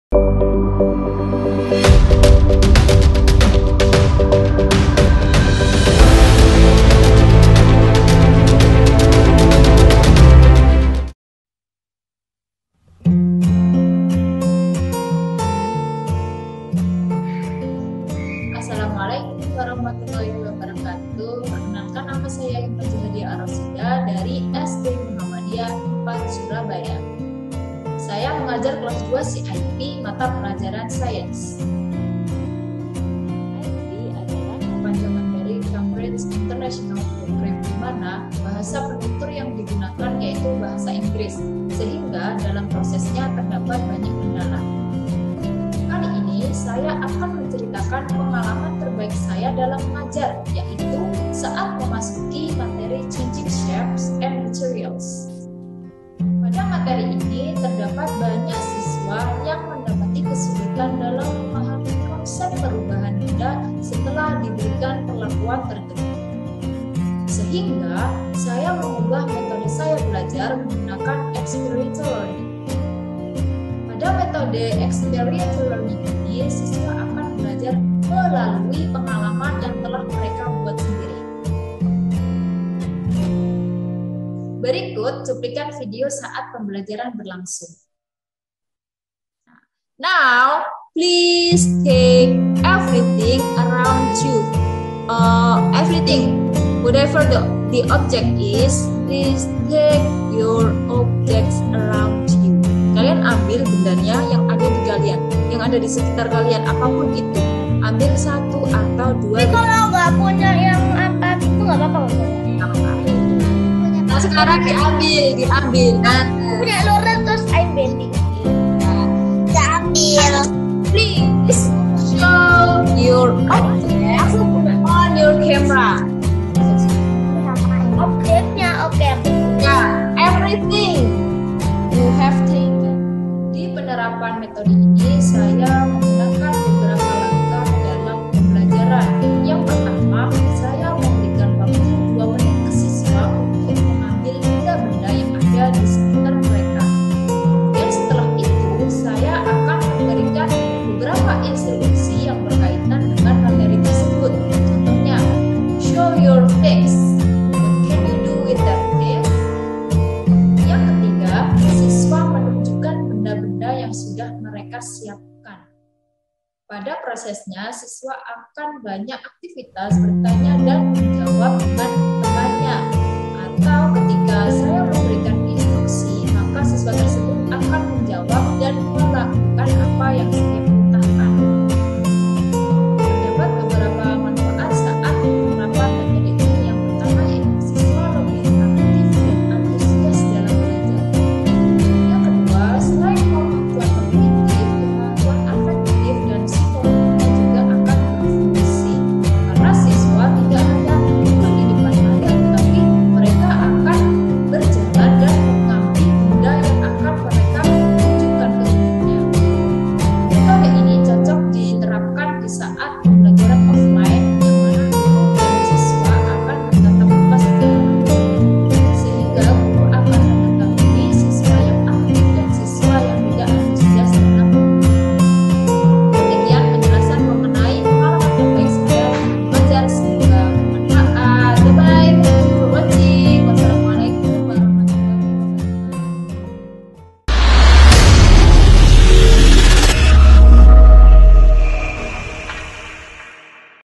Laporan Best Practice I Himma Jihadiah Ar Rosyidah, S Pd I SD Muhammadiyah 5 Surabaya
"PEMBENTUKAN KONSEP PERUBAHAN BENDA MENGGUNAKAN METODE "EXPERIMENTAL LEARNING"(LEARNING BY DOING) DI PEMBELAJARAN DI PEMBELAJARAN SAINS KELAS 2I SISWA SD MUHAMMADIYAH 4 SURABAYA"